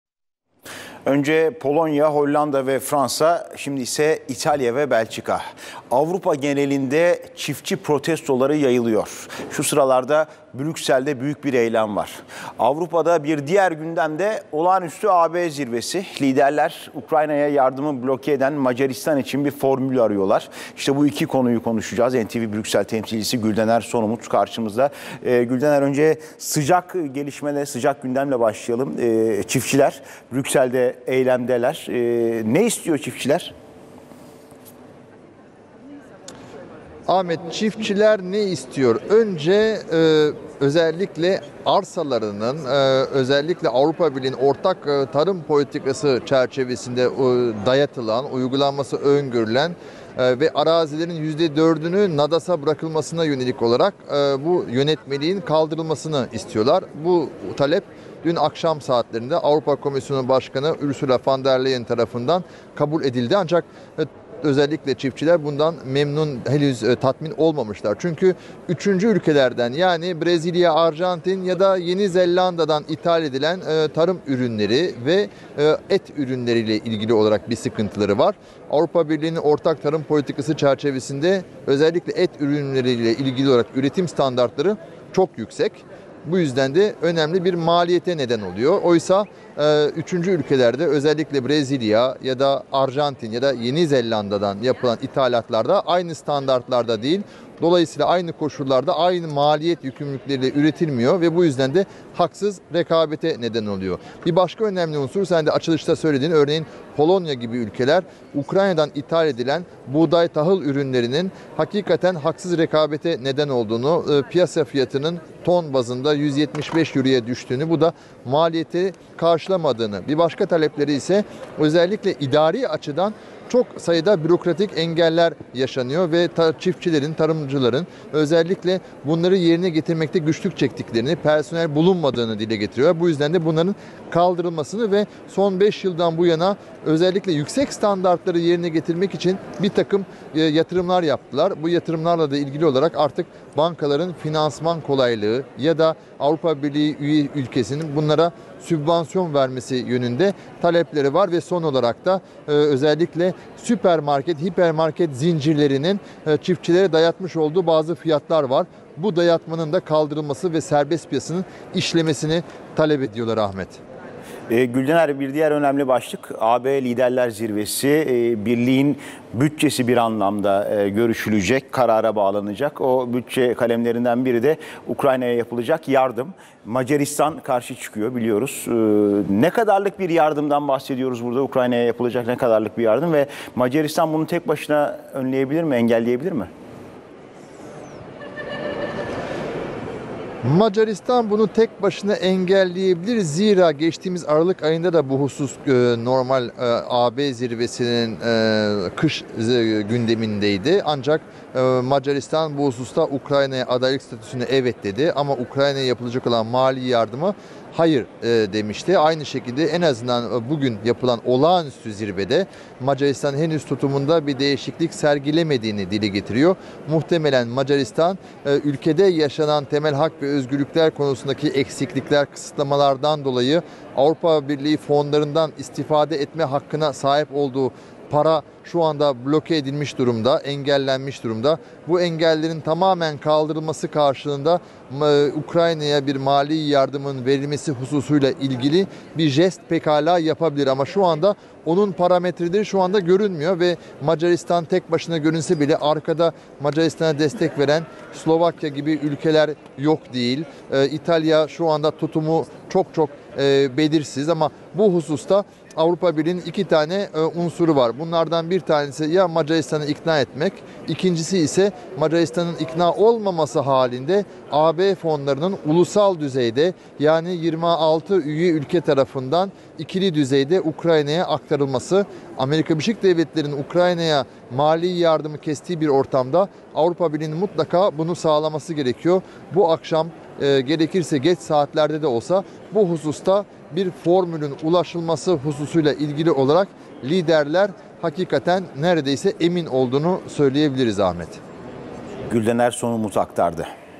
Avrupa Birliği Zirvesinde Traktörlü Protesto... | NTV
Hükümetin tarım politikalarına öfkeli Fransız çiftçilerin eylemi devam ediyor. Yüzlerce traktörle otobanları kapatan protestocular çok sayıda noktada trafiği kesmiş durumda. protestolara yanıt olarak Hükümetin attığı adımlar yeterli bulunmadı. "Dizel yakıta yönelik devlet yardımının devam edeceği" açıklamasına karşın, işçi sendikaları eylemlere devam kararı aldı.